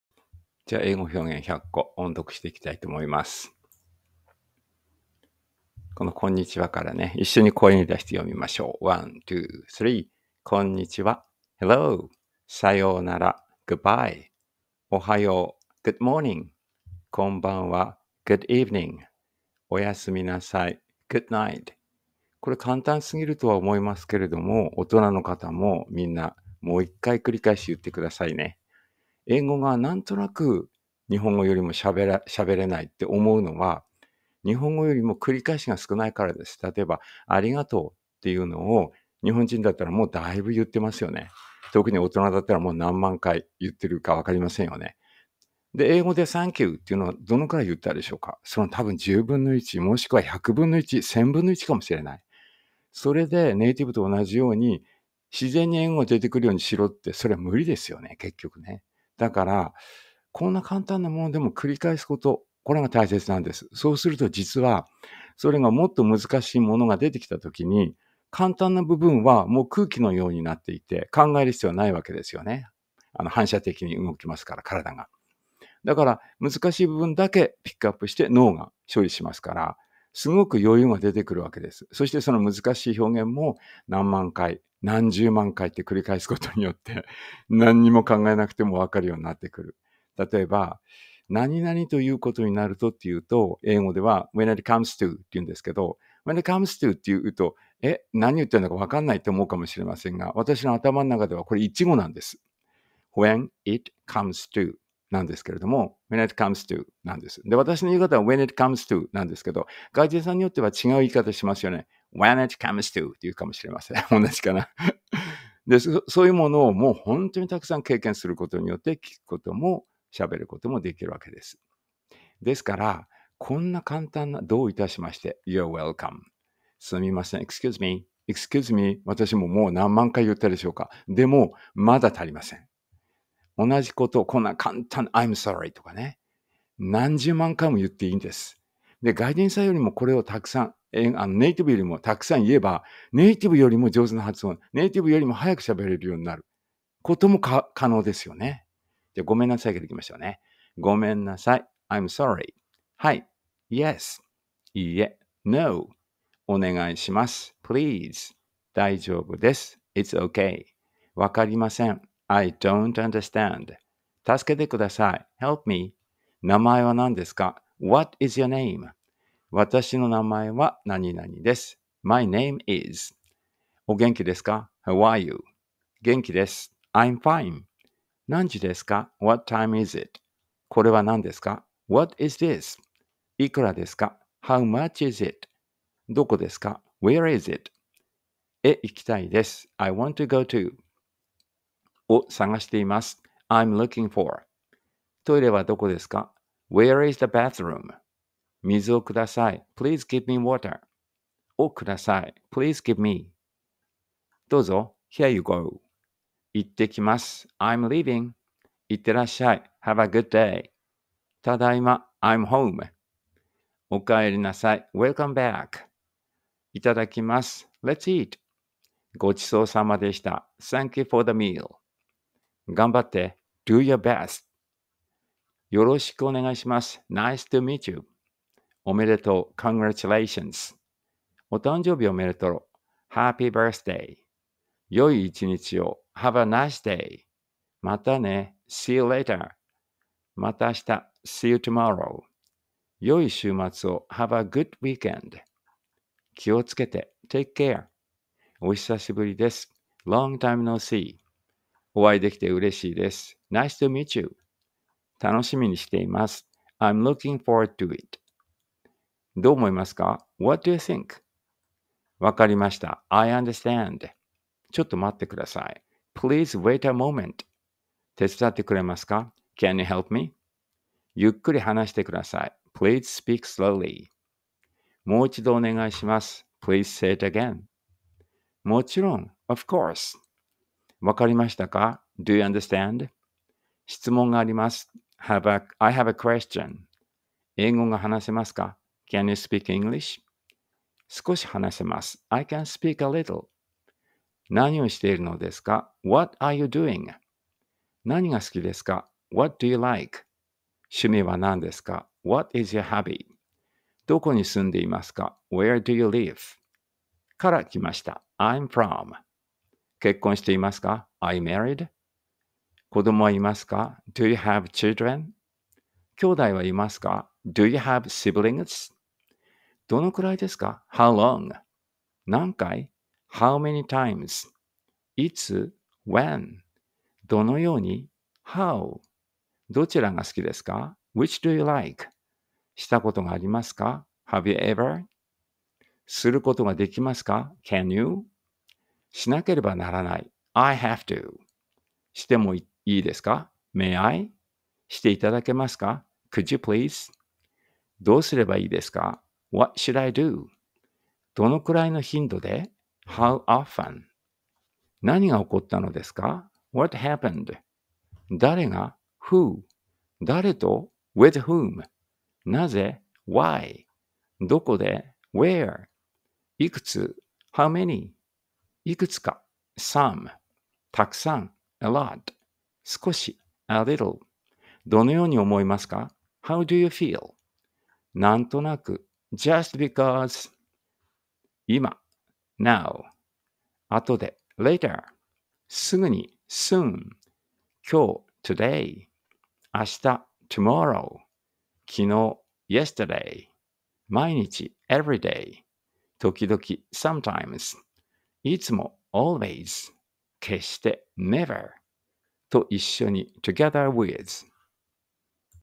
えいご おんどく 233
以下に英語表現100個とその日本語訳をリストアップしました。

1. こんにちは - Hello
2. さようなら - Goodbye
3. おはよう - Good morning
4. こんばんは - Good evening
5. おやすみなさい - Good night
6. ありがとう - Thank you
7. どういたしまして - You're welcome
8. すみません - Excuse me
9. ごめんなさい - I'm sorry
10. はい - Yes
11. いいえ - No
12. お願いします - Please
13. 大丈夫です - It's okay
14. わかりません - I don't understand
15. 助けてください - Help me
16. 名前は何ですか - What is your name?
17. 私の名前は〜です - My name is ~
18. お元気ですか - How are you?
19. 元気です - I'm fine
20. 何時ですか - What time is it?
21. これは何ですか - What is this?
22. いくらですか - How much is it?
23. どこですか - Where is it?
24. 〜へ行きたいです - I want to go to ~
25. 〜を探しています - I'm looking for ~
26. トイレはどこですか - Where is the bathroom?
27. 水をください - Please give me water
28. 〜をください - Please give me ~
29. どうぞ - Here you go
30. いってきます - I'm leaving
31. いってらっしゃい - Have a good day
32. ただいま - I'm home
33. おかえりなさい - Welcome back
34. いただきます - Let's eat
35. ごちそうさまでした - Thank you for the meal
36. がんばって - Do your best
37. よろしくお願いします - Nice to meet you
38. おめでとう - Congratulations
39. お誕生日おめでとう - Happy birthday
40. 良い一日を - Have a nice day
41. またね - See you later
42. また明日 - See you tomorrow
43. 良い週末を - Have a good weekend
44. 気をつけて - Take care
45. お久しぶりです - Long time no see
46. お会いできてうれしいです - Nice to meet you
47. 楽しみにしています - I'm looking forward to it
48. どう思いますか - What do you think?
49. わかりました - I understand
50. ちょっと待ってください - Please wait a moment
51. 手伝ってくれますか - Can you help me?
52. ゆっくり話してください - Please speak slowly
53. もう一度お願いします - Please say it again
54. もちろん - Of course
55. わかりましたか - Do you understand?
56. 質問があります - I have a question
57. 英語が話せますか - Can you speak English?
58. 少し話せます - I can speak a little
59. 何をしているのですか - What are you doing?
60. 何が好きですか - What do you like?
61. 趣味は何ですか - What is your hobby?
62. どこに住んでいますか - Where do you live?
63. 〜から来ました - I'm from ~
64. 結婚していますか - Are you married?
65. 子供はいますか - Do you have children?
66. 兄弟はいますか - Do you have siblings?
67. どのくらいですか - How long?
68. 何回 - How many times?
69. いつ - When?
70. どのように - How?
71. どちらが好きですか - Which do you like?
72. 〜したことがありますか - Have you ever ~?
73. 〜することができますか - Can you ~?
74. 〜しなければならない - I have to ~
75. 〜してもいいですか - May I ~?
76. 〜していただけますか - Could you please ~?
77. どうすればいいですか - What should I do?
78. どのくらいの頻度で - How often?
79. 何が起こったのですか - What happened?
80. 誰が - Who?
81. 誰と - With whom?
82. なぜ - Why?
83. どこで - Where?
84. いくつ - How many?
85. いくつか - Some
86. たくさん - A lot
87. 少し - A little
88. どのように思いますか - How do you feel?
89. なんとなく - Just because
90. 今 - Now
91. 後で - Later
92. すぐに - Soon
93. 今日 - Today
94. 明日 - Tomorrow
95. 昨日 - Yesterday
96. 毎日 - Every day
97. 時々 - Sometimes
98. いつも - Always
99. 決して - Never
100. 〜と一緒に - Together with ~